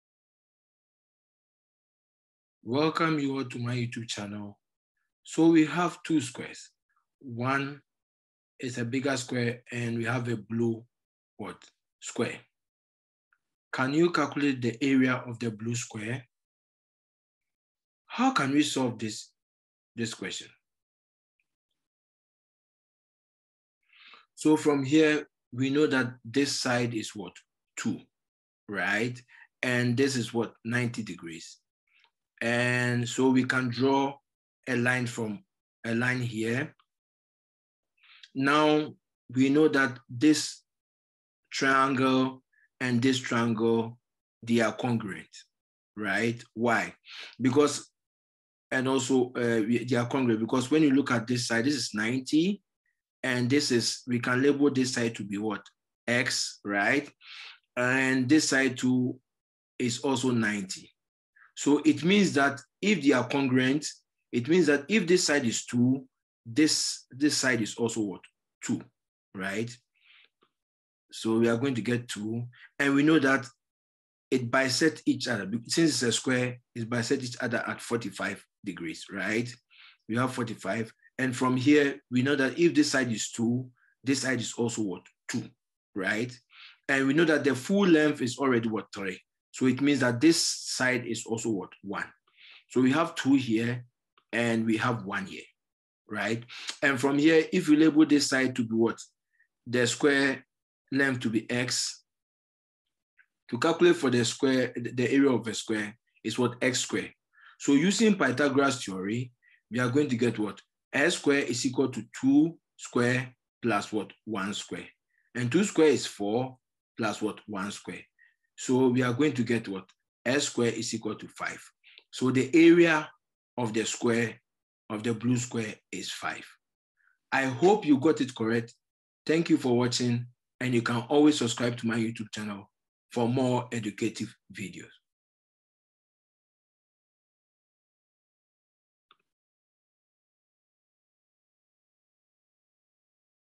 Two squares geometry puzzle.Could you find the blue area?
Geometry, Polynomial expressions, sum of squares, algebra, algebraic equations, substitution, Challenging Math Problems,  polynomial equations, algebra challenge, algebraic identities,  algebraic challenge.